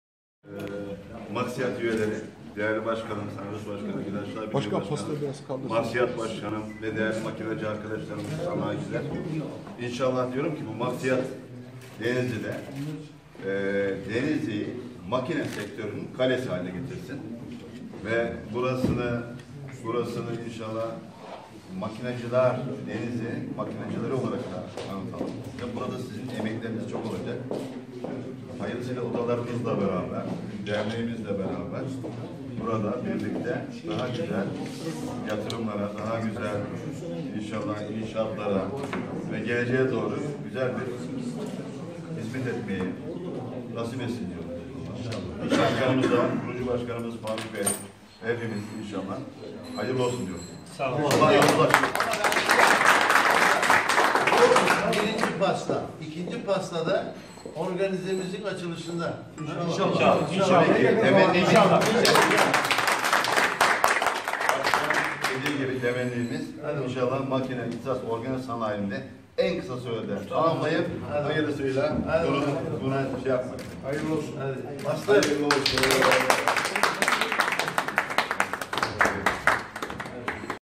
Maksiad ilk pastasını kesti
Maksiad Pastayı Denib Bşk Süleyman Kocasert, Denizli Mv. Şahin Tin, Maksiad Bşk. Mehmet Sarı, Dso Bşk. Müjdat Keçeci ile birlikte kesti